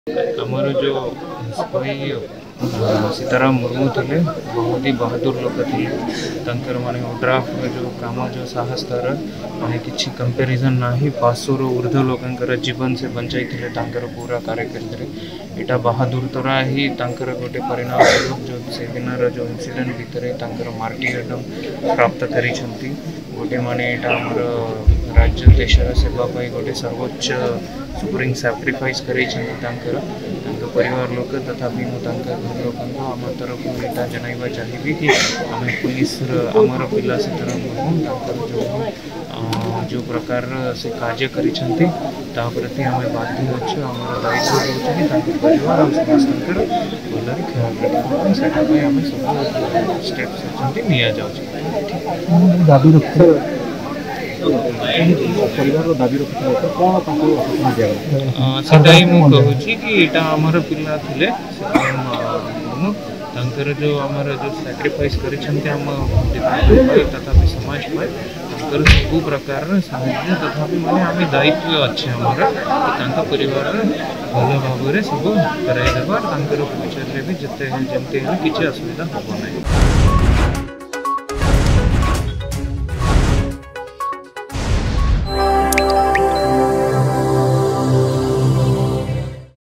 ODRAF ଯବାନ ସୀତାରାମ ମୁର୍ମୁଙ୍କ ମୃତ୍ୟ ପରେ SP ଙ୍କ ପ୍ରତିକ୍ରିୟା
ODRAF ଯବାନ ସୀତାରାମ ମୁର୍ମୁଙ୍କ ମୃତ୍ୟ ପରେ ମୟୁରଭଞ୍ଜ SP ଙ୍କ ପ୍ରତିକ୍ରିୟା